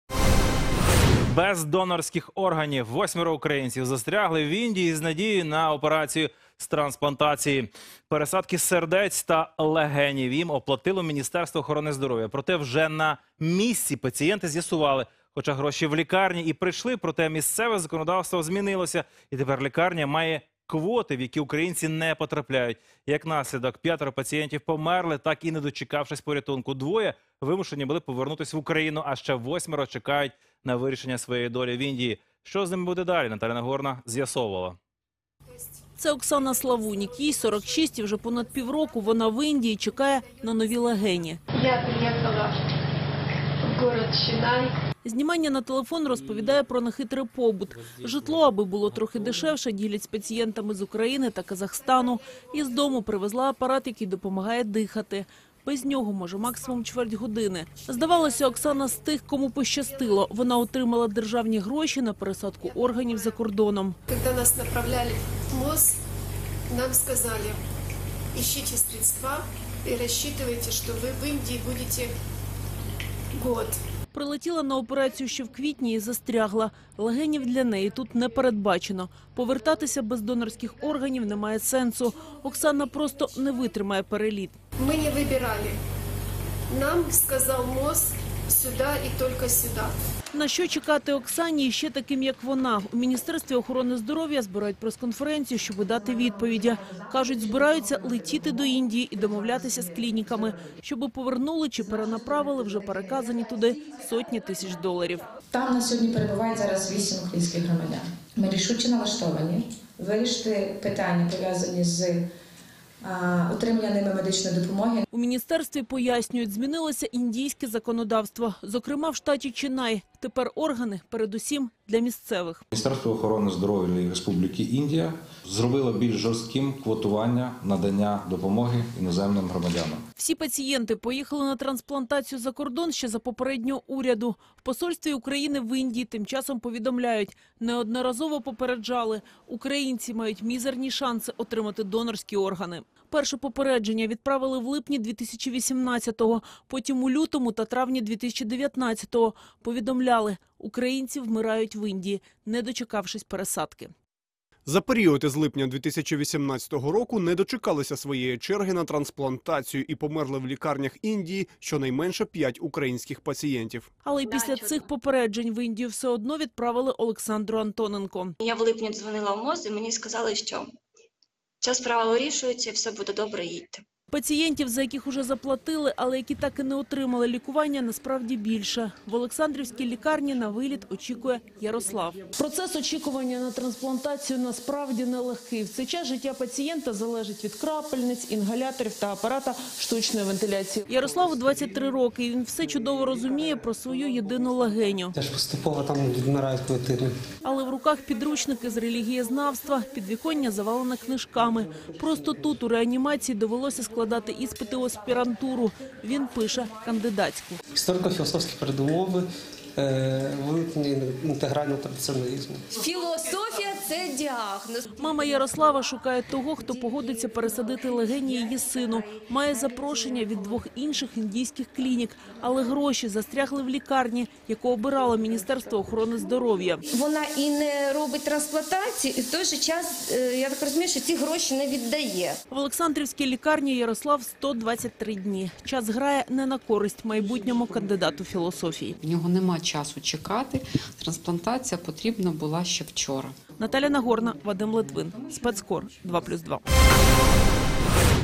У лікарнях Індії без можливості трансплантації вмирають українці
Пересадки сердець та легенів їм оплатило Міністерство охорони здоров'я, проте вже на місці пацієнти з’ясували, що, хоча гроші в лікарні й прийшли, місцеве законодавство змінилося. Тому тепер лікарня має квоти, в які українці не потрапляють. В результаті 5 пацієнтів померли, так і не дочекавшись порятунку, двоє вимушені були повернутися в Україну, а ще восьмеро чекають на вирішення своєї долі в Індії. Що з ними буде далі, дізнавалася Наталя Нагорна.